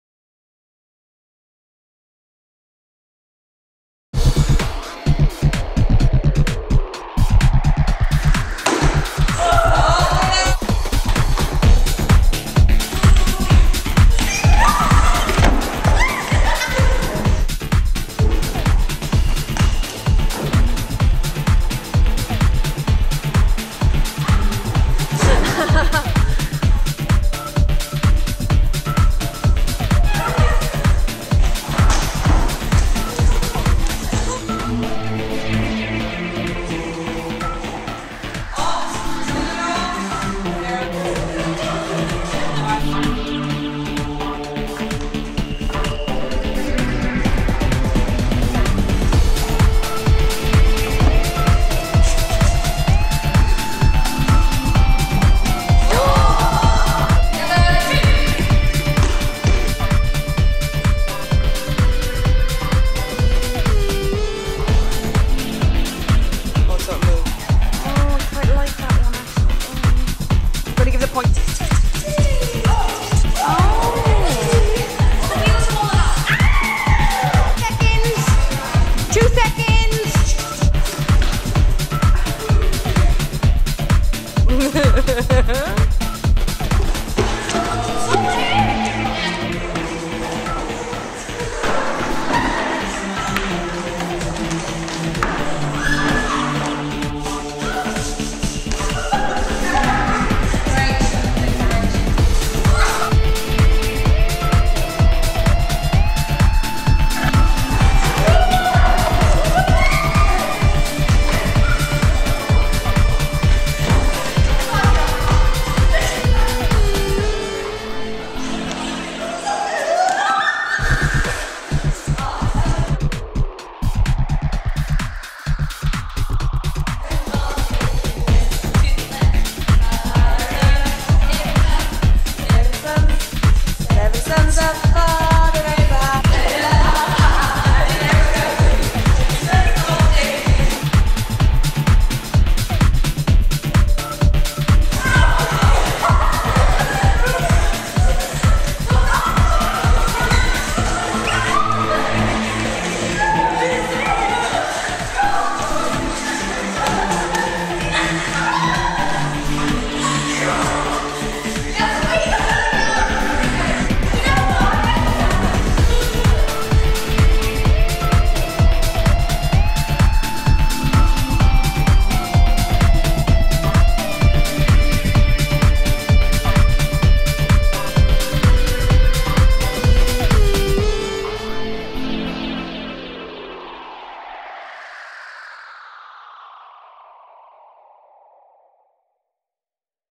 Young Women's Arts Award at Eccles Youth Centre: The Games
A few clips of the games we played in drama to energise the group, improve team work and confidence and to warm up for performing.